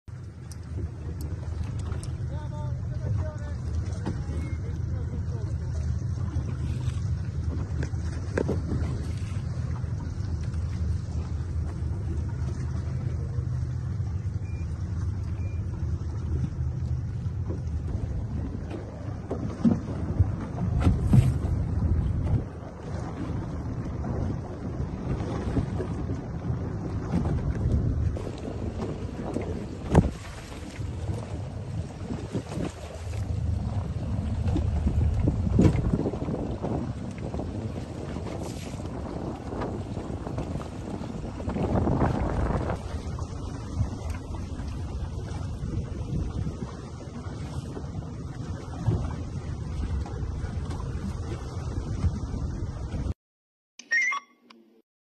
Naufragio a Porticello, proseguite fino a tarda notte le ricerche dei sei dispersi del Bayesian
(Agenzia Vista) Sono proseguite per tutta la giornata per poi chiudersi in serata le ricerche dei dispersi del Bayesian, la nave da 56 metri affondata a Porticello durante una tempesta. Le ricerche sono poi riprese la mattina successiva.